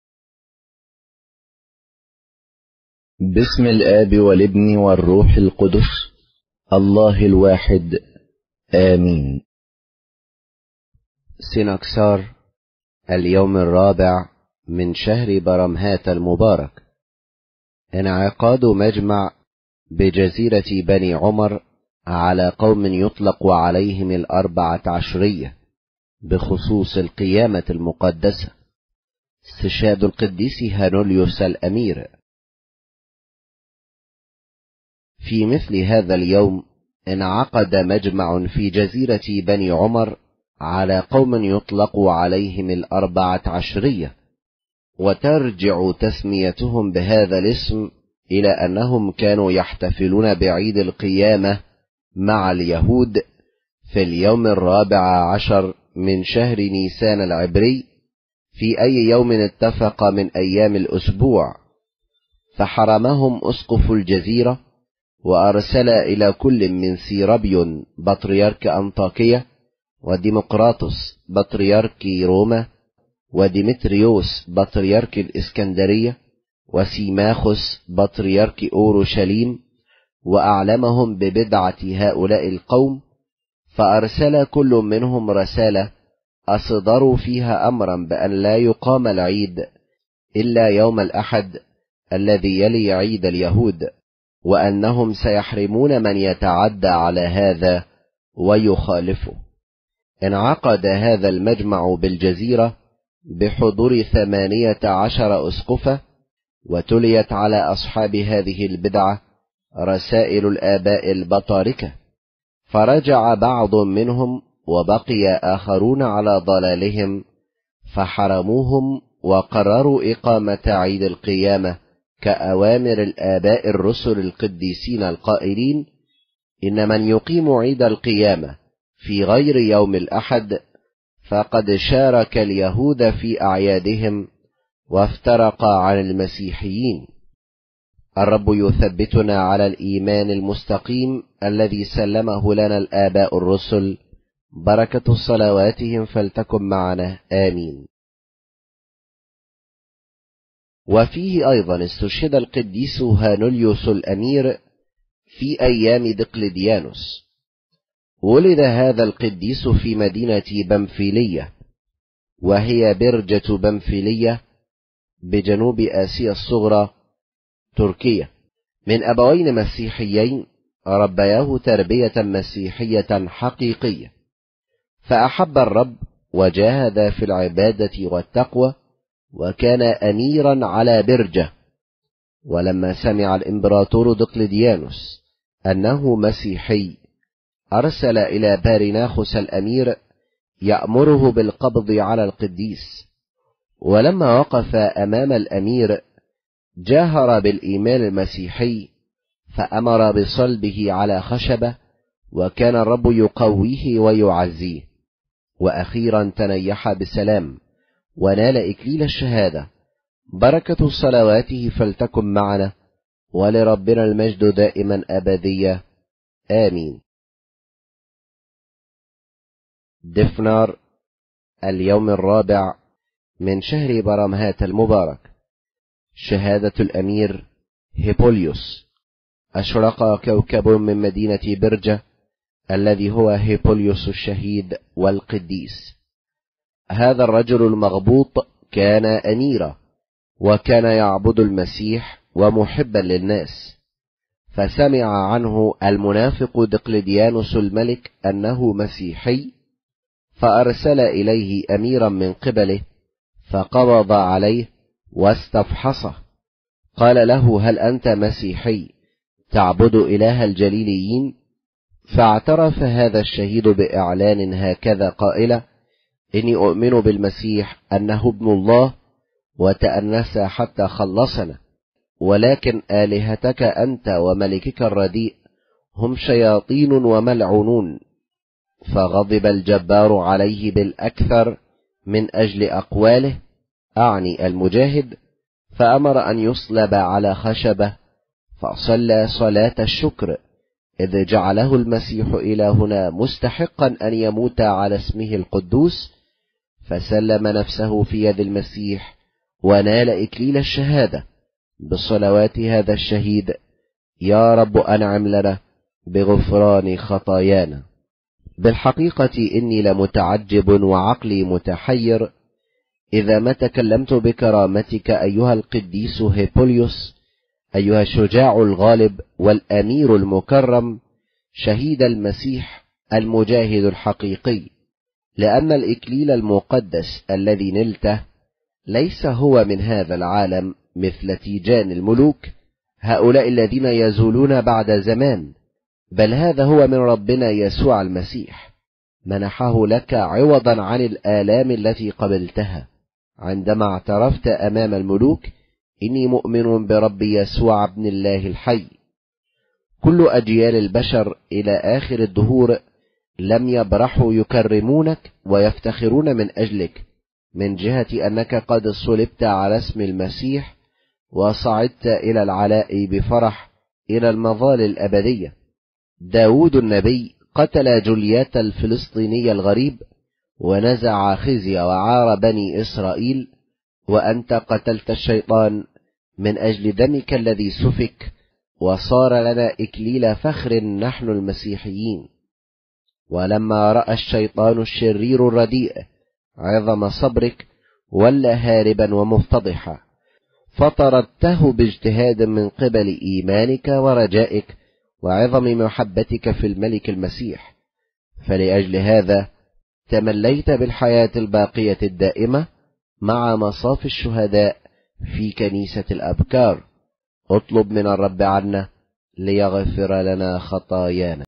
سنكسار اليوم ٤ برمهات
مجمع الاربعه عشريه بجزيره عمر
 استشهاد القديس هانوليوس الامير